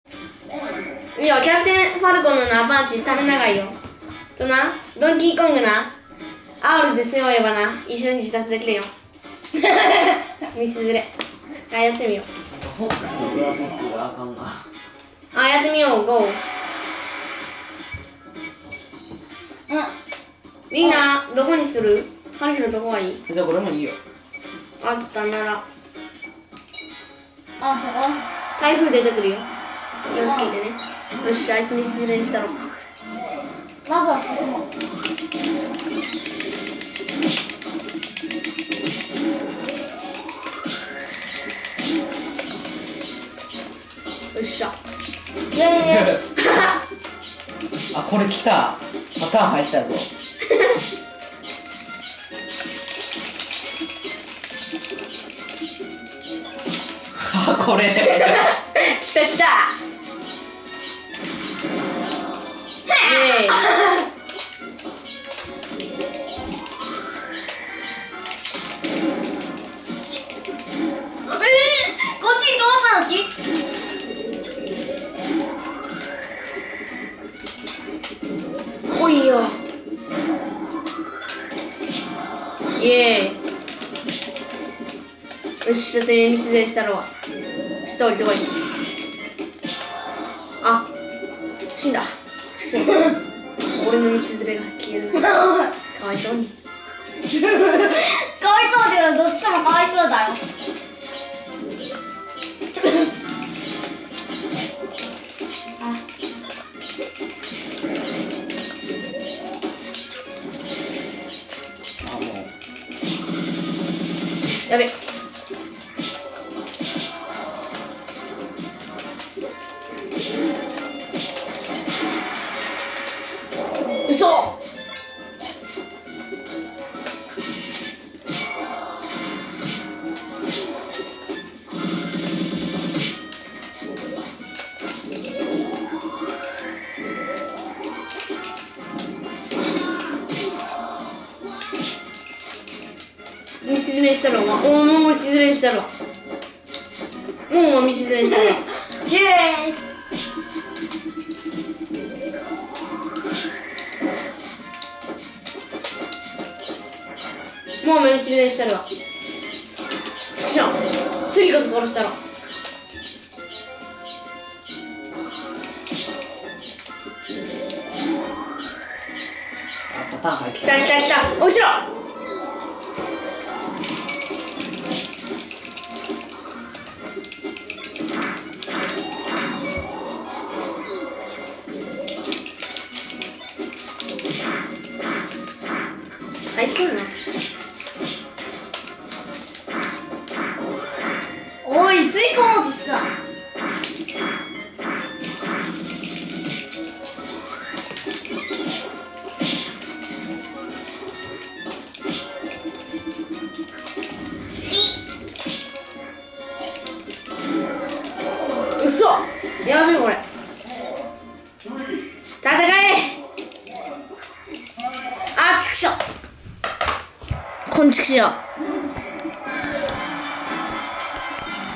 プレイヤー３人ＶＳレベル９コンピューターサムス
全員玄人。コンピューターは、レベル９のサムス。レベル９・・・危うし。